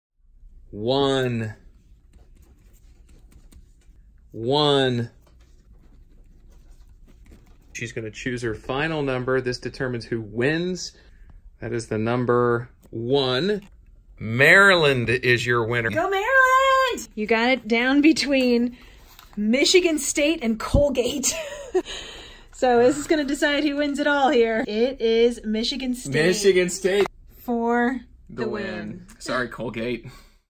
Bird Box Brackets. We filled out our March Madness brackets blind-folded!
Honestly, we have no ides what we're doing when it comes to choosing our picks for March Madness so we decided to do it blind-folded.  See how it turned out!